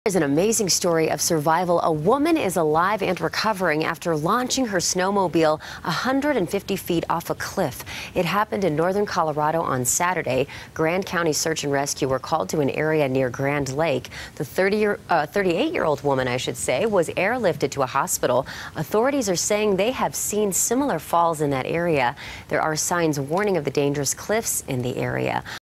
Woman Survives Snowmobile Fall Off Cliff
A snowmobiler was seriously injured when she went 150 feet down a cliff near Grand Lake on Sunday.